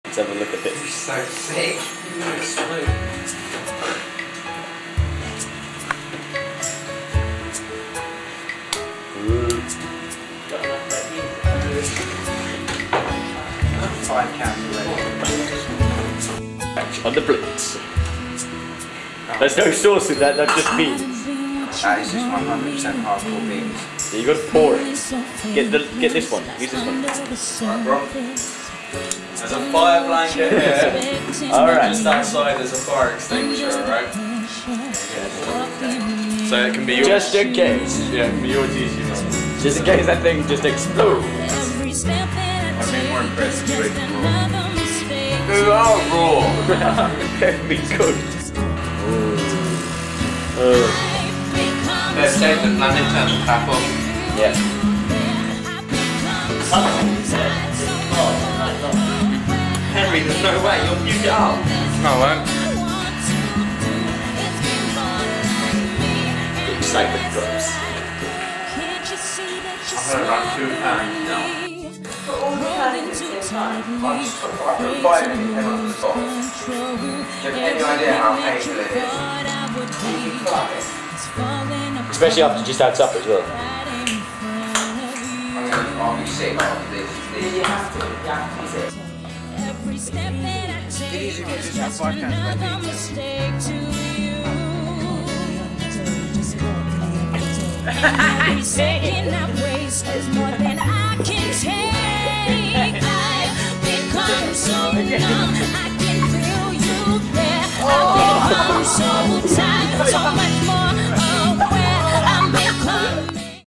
Union Jackass Baked Bean Challenge
How many cans of Iceland value beans can you eat?